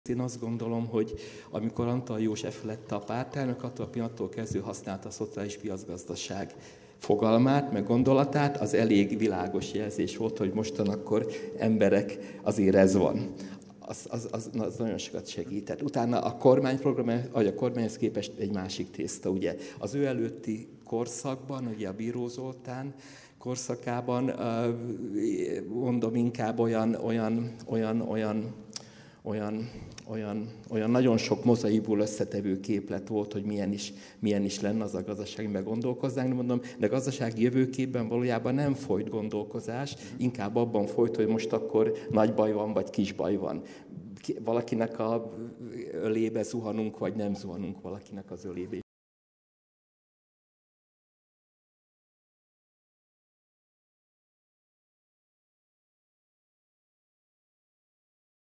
Nagy Zoltán az MDF Antall előtti gazdasági jövőképéről
"Bíró Zoltán korszakában. gazdasági jövőképben valójában nem folyt gondolkozás. Inkább abban folyt, hogy nagy baj van, vagy kis baj van."

Dr. Nagy Zoltán az MDF Antall előtti gazdasági jövőképéről. A Modell és Valóság előadássorozat IX. évadának "Gazdasági jövőképek 1990" című rendezvényére 2020. február 13-án került sor, Lesti Árpád moderálása mellett.